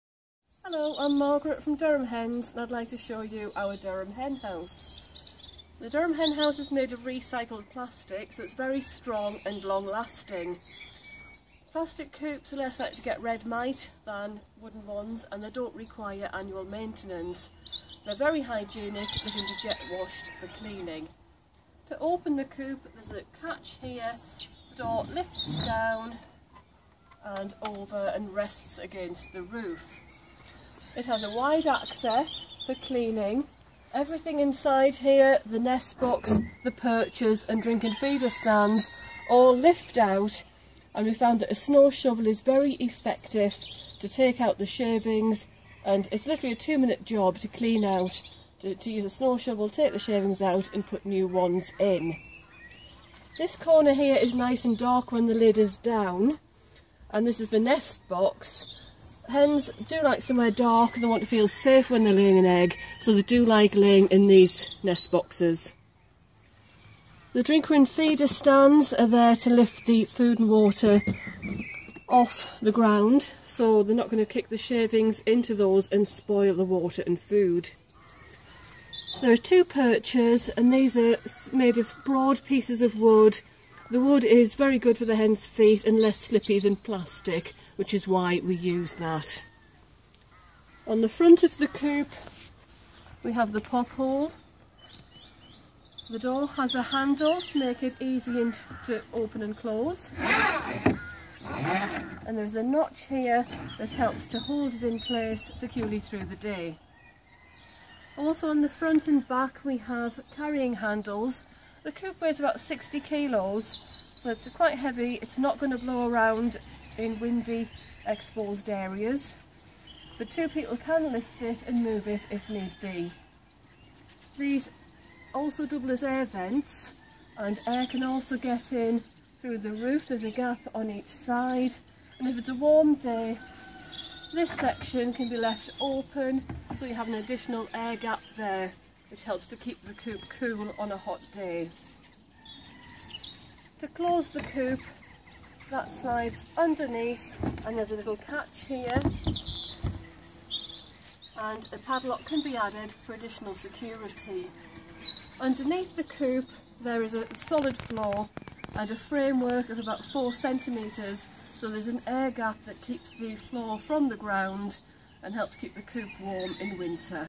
Durham Hen House - A Practical Plastic Coop
Durham Hen House is a practical chicken coop made from recycled plastic. Watch this demo for more information.